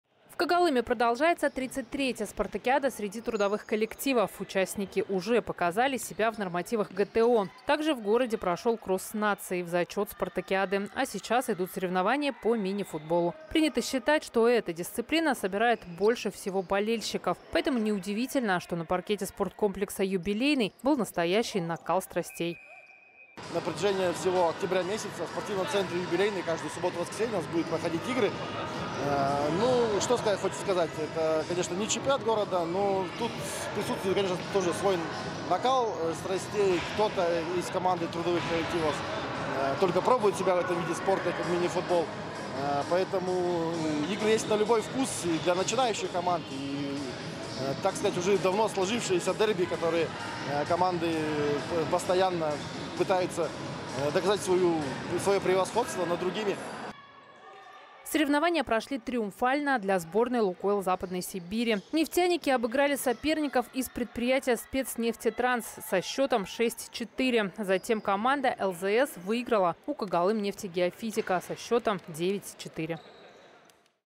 Соревнования по мини-футболу в Когалыме
Продолжается 33-я Спартакиада среди трудовых коллективов